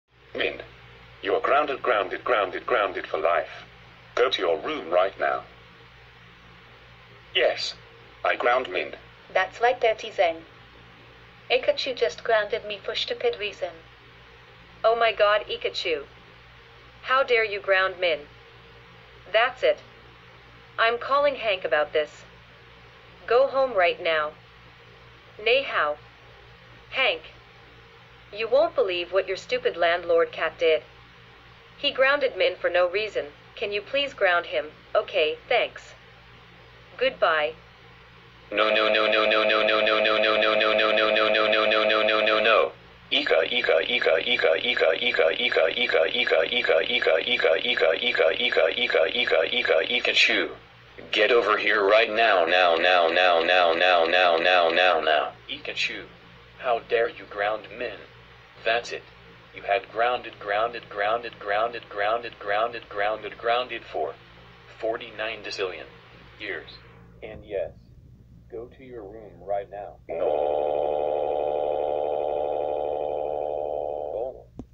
Ika Chu (Paws of Fury: The Legend of Hank) grounds Min (Kung Fu Wa) and gets grounded
Cast:
Hila as Min
Carlos as Ika Chu's crying voice